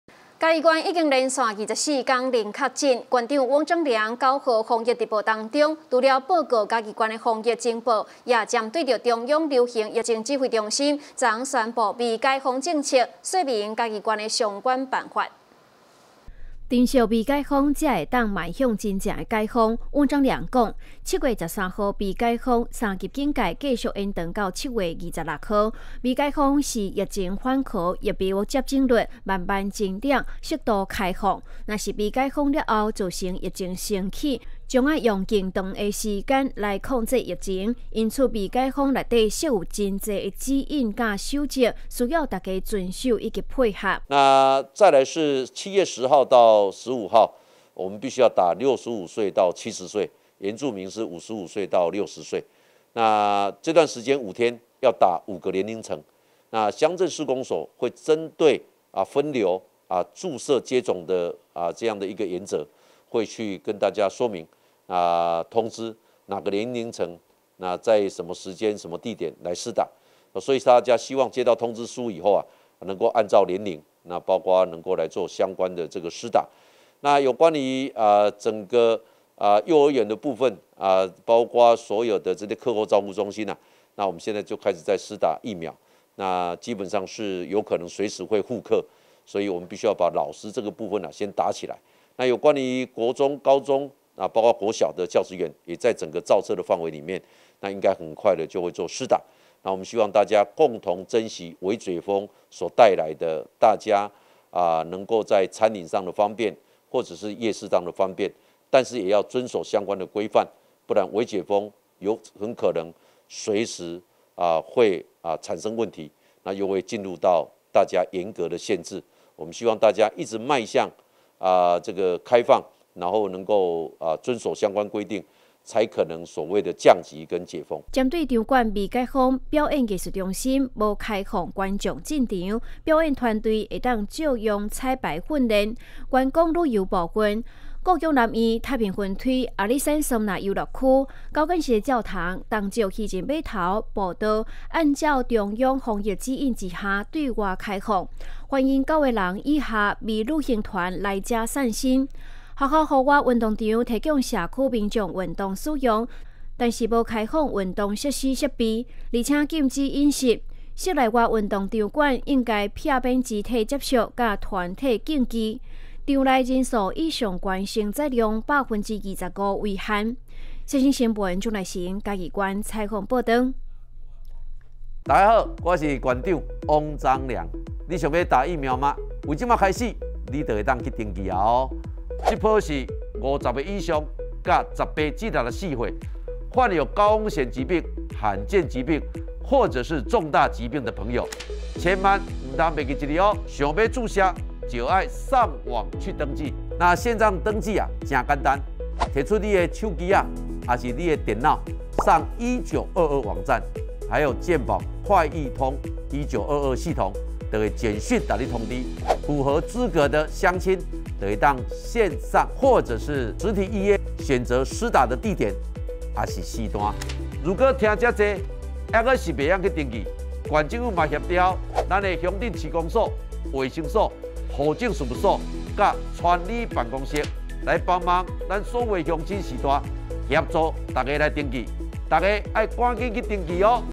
世新新聞 防疫快報！ 微解封 嘉義縣落實中央防疫指引
110年07月10日 (六) 世新新聞

防疫快報！ 微解封 嘉義縣落實中央防疫指引

嘉義縣已連續第24天0確診，縣長翁章梁9日防疫直播中，除報告嘉義縣防疫情報，也針對中央流行疫情指揮中心昨日宣布微解封政策，說明嘉義縣相關辦法。